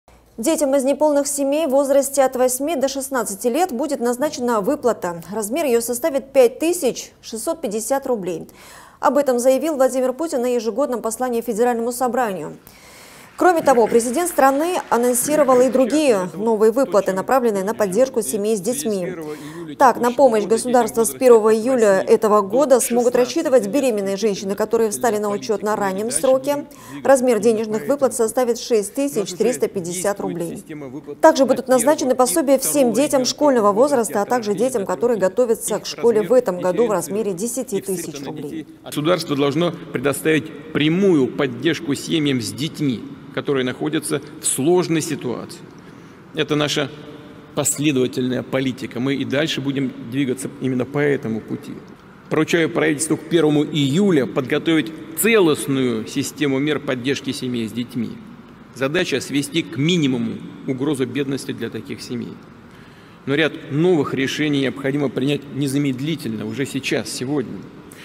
Владимир Путин анонсировал новые меры поддержки семей с детьми
Детям из неполных семей в возрасте от 8 до 16 лет будет назначена выплата. Размер ее составит 5 650 рублей. Об этом заявил Владимир Путин на ежегодном послании Федеральному собранию.

Кроме того, президент страны анонсировал и другие новые выплаты, направленные на поддержку семей с детьми. Так, на помощь государства с 1 июля 2021 года смогут рассчитывать беременные женщины, которые встали на учет на раннем сроке. Размер денежных выплат составит 6350 рублей. Также будут назначены пособия всем детям школьного возраста, а также детям, которые готовятся к школе в этом году в размере 10 тысяч рублей.